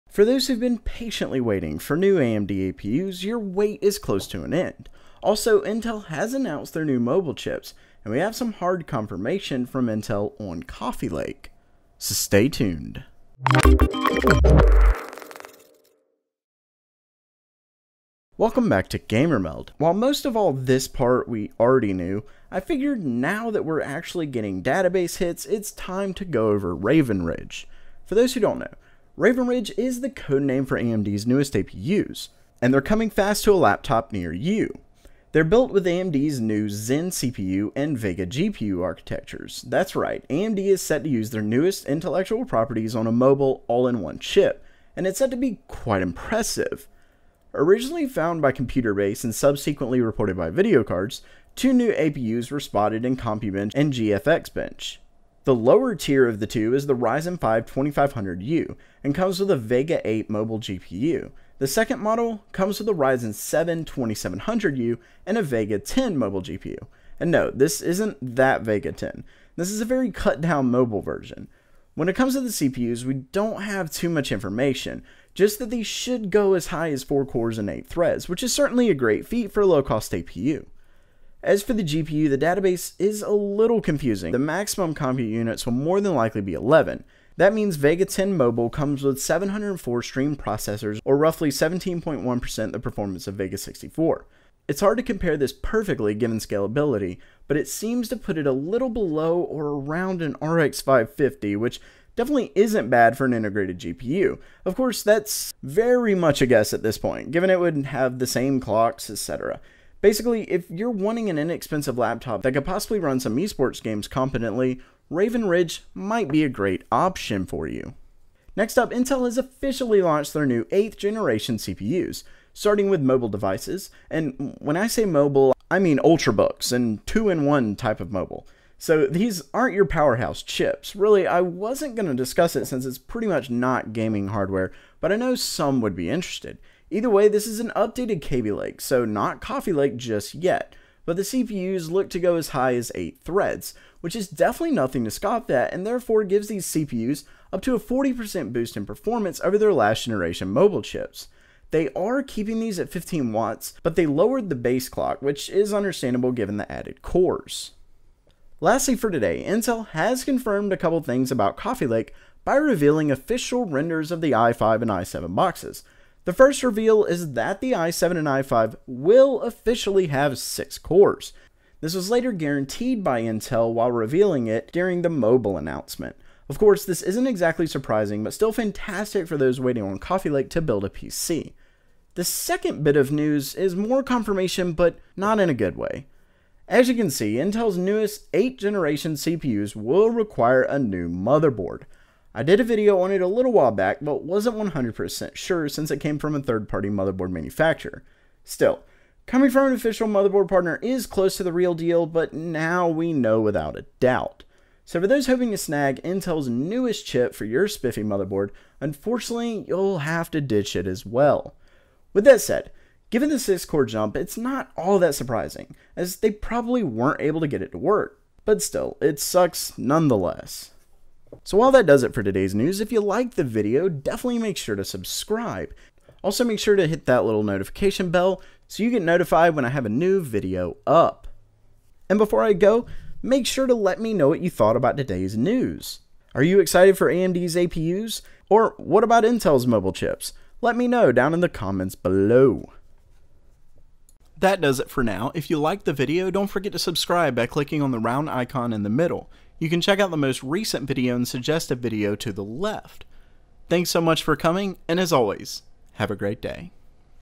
Ryzen & Vega APUs Spotted + Coffee Lake Confirmation!
We have confirmation on Intel's Coffee Lake News and AMD's APUs!